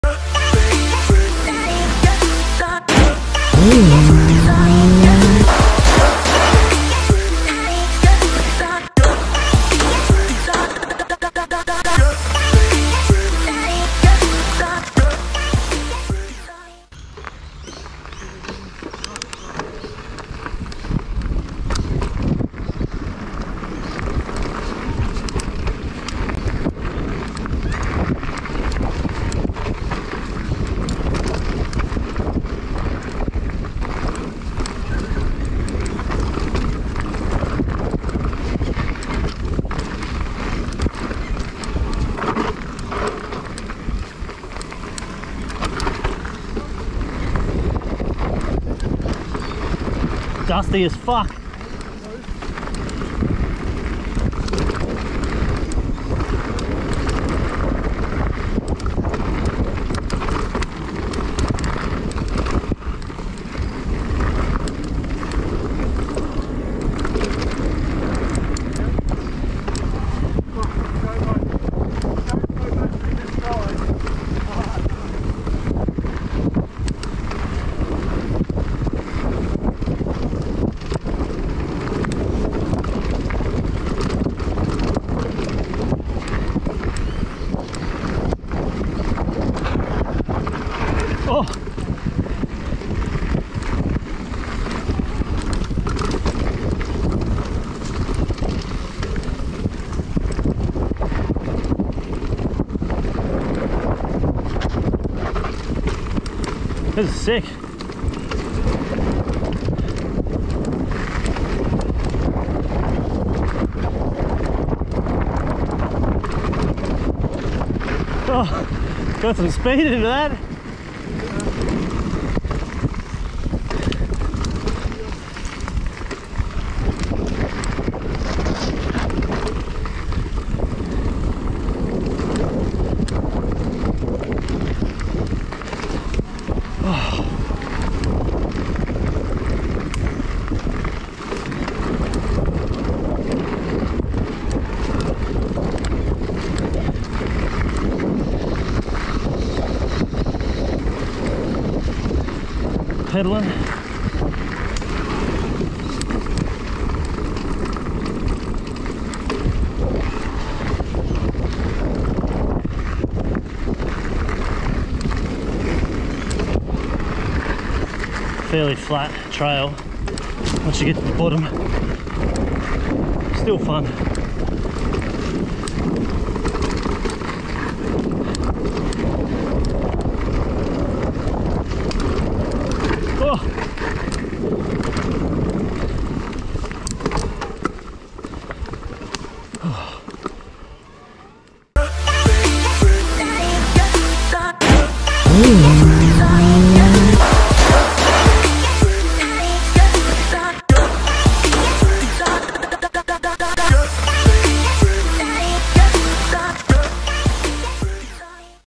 Rocket Frog | raw edit | Gap Creek
Welcome back the Bikes And Bullets YouTube channel. Im uploading a series of small raw edits showcasing a particular trail where I take what I believe are the best bits of footage and  piece them together in to one singular video, with no background music, just the sound of the rubber on the dirt.